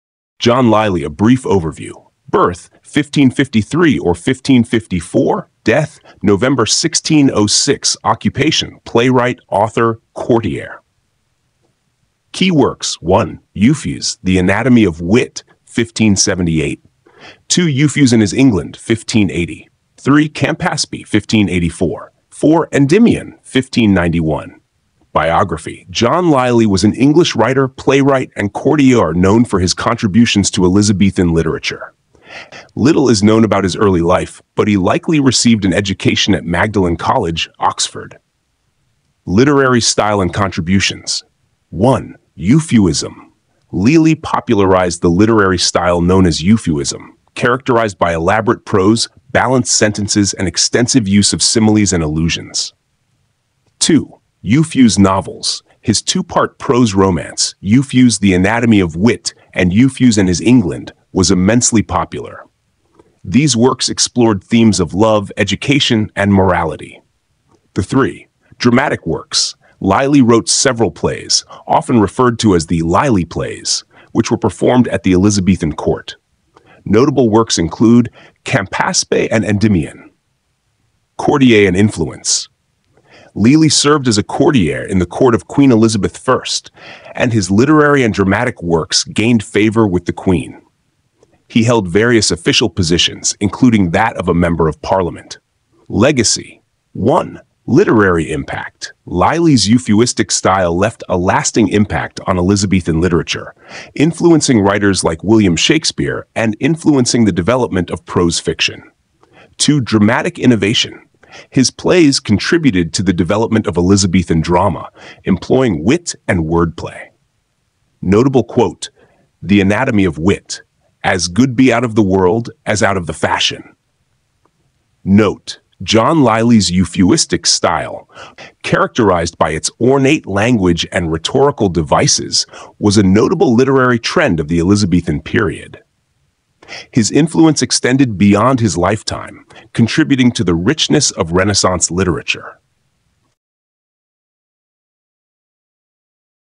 John Lyly | Biography and famous works of John Lyly | Who was John Lyly?
**John Lyly: A Brief Overview**

**Birth:** 1553 or 1554

**Death:** November 1606

**Occupation:** Playwright, Author, Courtier

**Key Works:**
1. *Euphues: The Anatomy of Wit* (1578)
2. *Euphues and His England* (1580)
3. *Campaspe* (1584)
4. *Endymion* (1591)

**Biography:**
John Lyly was an English writer, playwright, and courtier known for his contributions to Elizabethan literature. Little is known about his early life, but he likely received an education at Magdalen College, Oxford.

**Literary Style and Contributions:**
1. **Euphuism:** Lyly popularized the literary style known as Euphuism, characterized by elaborate prose, balanced sentences, and extensive use of similes and allusions.
2. **Euphues Novels:** His two-part prose romance, *Euphues: The Anatomy of Wit* and *Euphues and His England*, was immensely popular. These works explored themes of love, education, and morality.
3. **Dramatic Works:** Lyly wrote several plays, often referred to as the "Lyly plays," which were performed at the Elizabethan court. Notable works include *Campaspe* and *Endymion*.

**Courtier and Influence:**
Lyly served as a courtier in the court of Queen Elizabeth I, and his literary and dramatic works gained favor with the queen. He held various official positions, including that of a member of Parliament.

**Legacy:**
1. **Literary Impact:** Lyly's Euphuistic style left a lasting impact on Elizabethan literature, influencing writers like William Shakespeare and influencing the development of prose fiction.
2. **Dramatic Innovation:** His plays contributed to the development of Elizabethan drama, employing wit and wordplay.

**Notable Quote (from *Euphues: The Anatomy of Wit*):**
"As good be out of the World, as out of the Fashion."

**Note:** John Lyly's Euphuistic style, characterized by its ornate language and rhetorical devices, was a notable literary trend of the Elizabethan period. His influence extended beyond his lifetime, contributing to the richness of Renaissance literature.